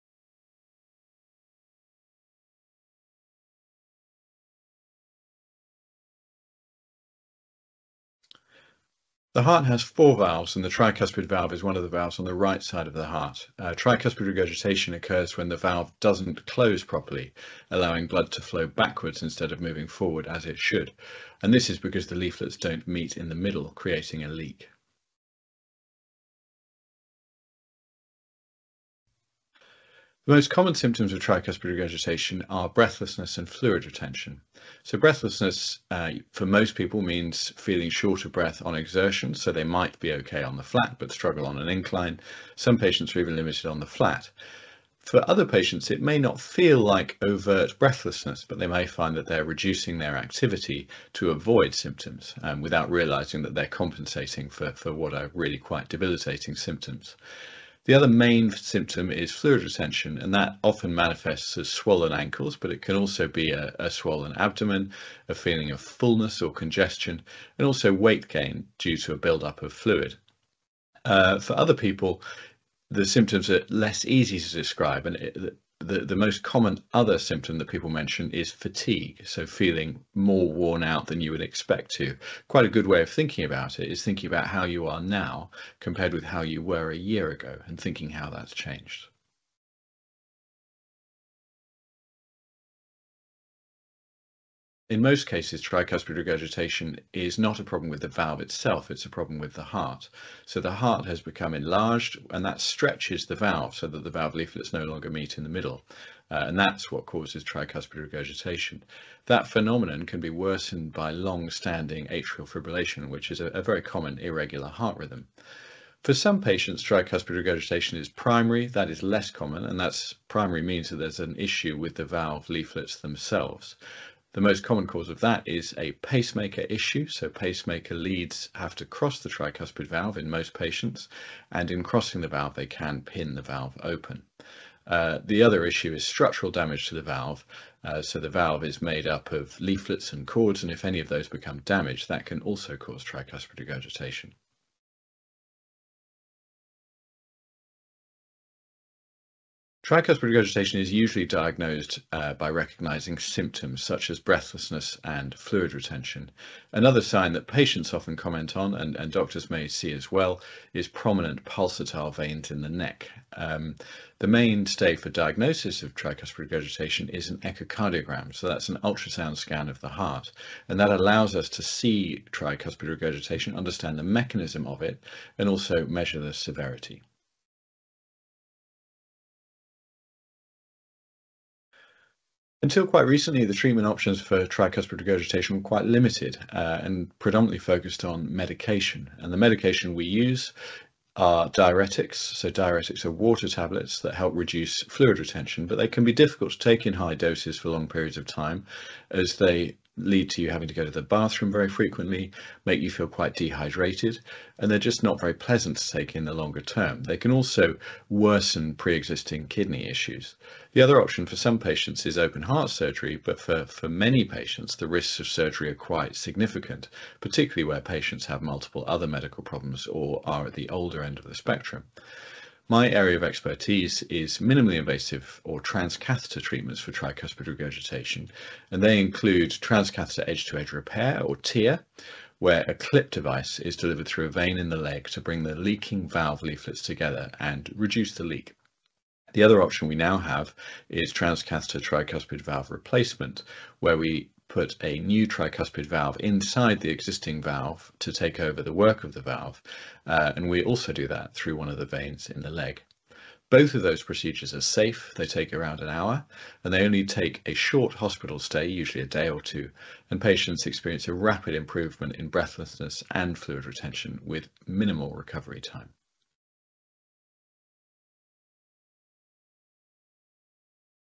What to know about tricuspid regurgitation
In this video, consultant cardiologist Dr Sam Dawkins tells us all about tricuspid regurgitation, such as what causes it, and the different treatment options that patients can explore.

00:00 What is tricuspid regurgitation?´
00:26 What are the symptoms of tricuspid regurgitation?
01:29 What causes tricuspid regurgitation?
02:25 How is tricuspid regurgitation diagnosed?
02:58 What are the treatment options for tricuspid regurgitation?